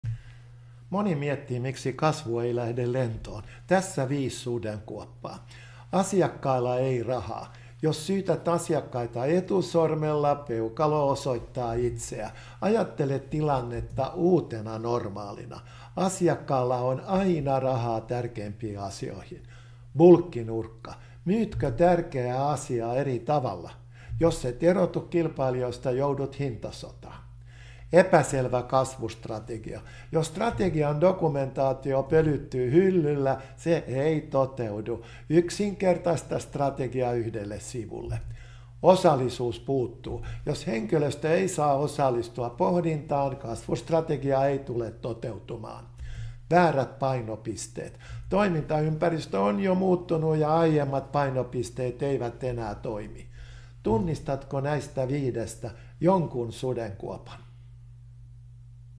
5 kasvun sudenkuoppaa
Moni miettii miksi kasvu ei lähde lentoon. Tässä 5 sudenkuoppaa.

1. Asiakkailla ei rahaa. Jos syytät asiakkaita etusormella, peukalo osoittaa itseä. Ajattele tilannetta uutena normaalina. Asiakkaalla on aina rahaa tärkeimpiin asioihin.
2. Bulkkinurkka. Myytkö tärkeää asiaa eri tavalla? Jos et erotu kilpailijoista, joudut hintasotaan.
3. Epäselvä kasvustrategia. Jos strategian dokumentaatio pölyttyy hyllyllä, se ei toteudu. Yksinkertaista strategia yhdelle sivulle.
4. Osallisuus puuttuu. Jos henkilöstö ei saa osallistua pohdintaan, kasvustrategia ei tule toteutumaan.
5. Väärät painopisteet. Toimintaympäristö on jo muuttunut ja aiemmat painopisteet eivät enää toimi.

Tunnistatko näistä viidestä jonkun sudenkuopan?